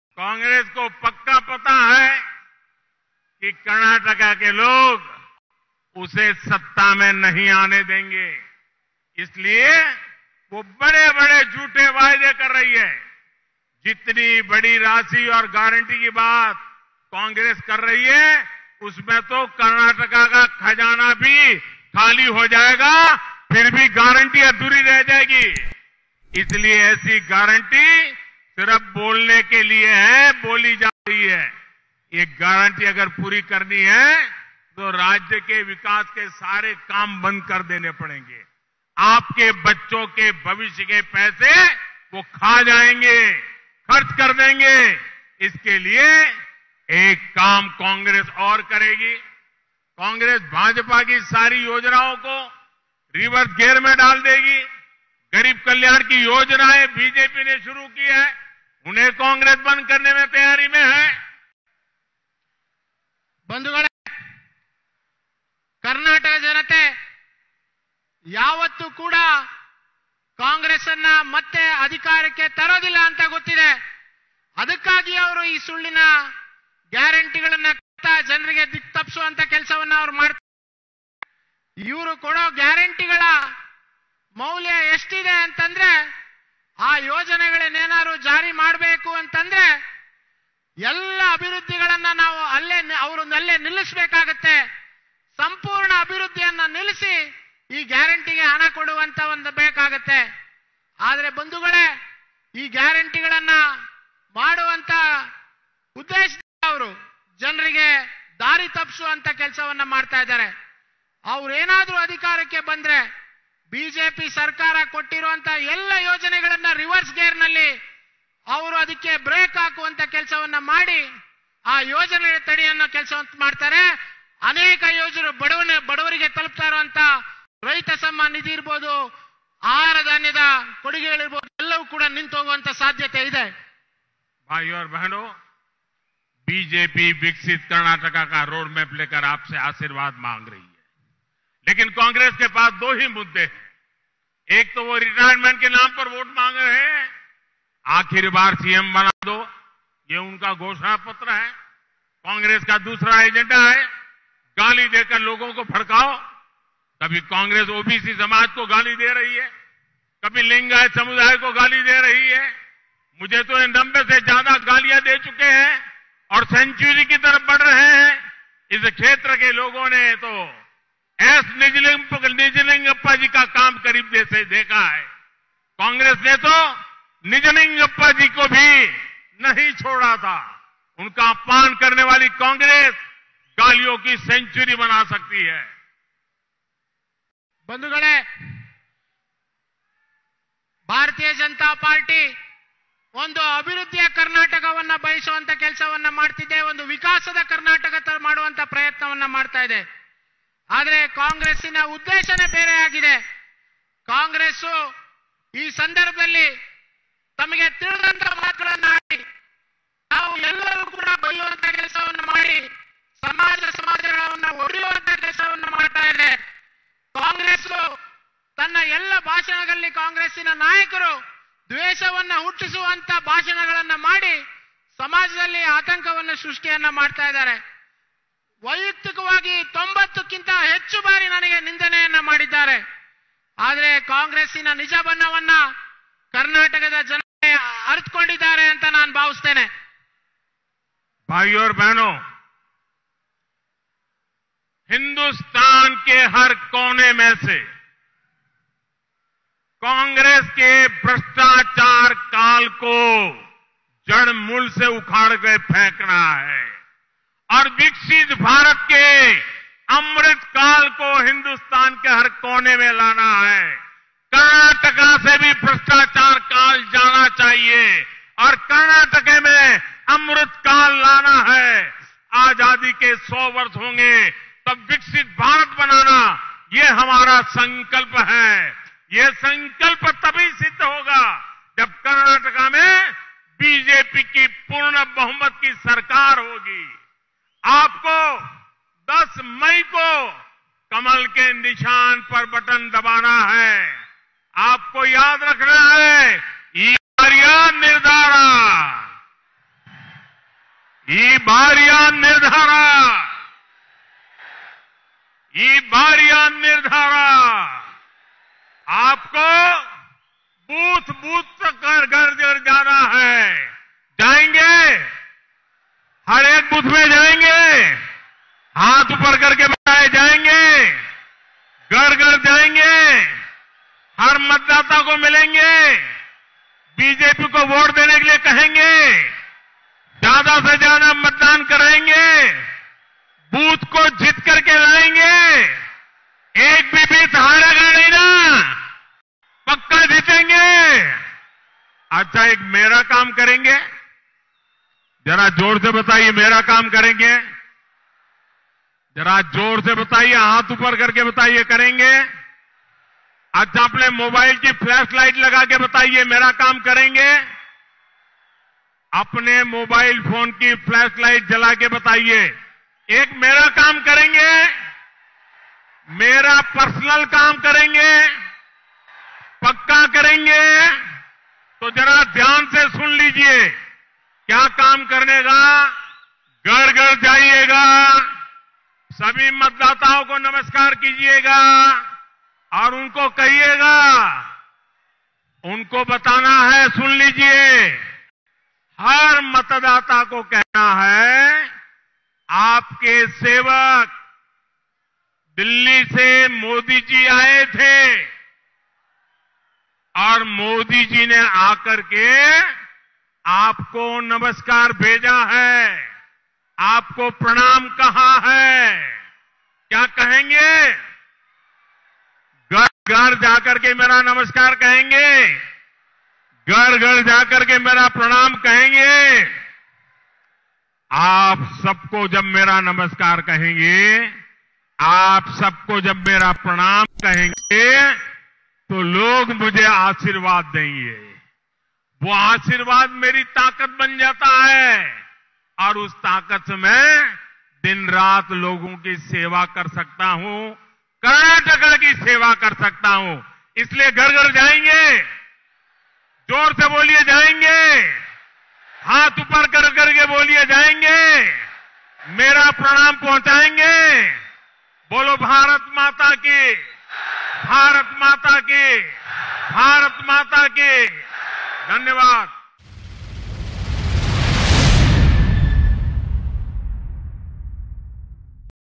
PM Modi Strikes At Congress | ಕಾಂಗ್ರೆಸ್​ ಅಧಿಕಾರಕ್ಕೆ ಬಂದರೆ ರಾಜ್ಯದ ಬೊಕ್ಕಸವೆಲ್ಲ ಖಾಲಿ ಆಗತ್ತೆ
PM Modi Strikes At Congress |  ಕಾಂಗ್ರೆಸ್​ ಅಧಿಕಾರಕ್ಕೆ ಬಂದರೆ ರಾಜ್ಯದ ಬೊಕ್ಕಸವೆಲ್ಲ ಖಾಲಿ ಆಗತ್ತೆ. PM Narendra Modi Strikes At Congress In Chitradurga |  ಕಾಂಗ್ರೆಸ್​ ಅಧಿಕಾರಕ್ಕೆ ಬಂದರೆ ರಾಜ್ಯದ ಬೊಕ್ಕಸವೆಲ್ಲ ಖಾಲಿ ಆಗತ್ತೆ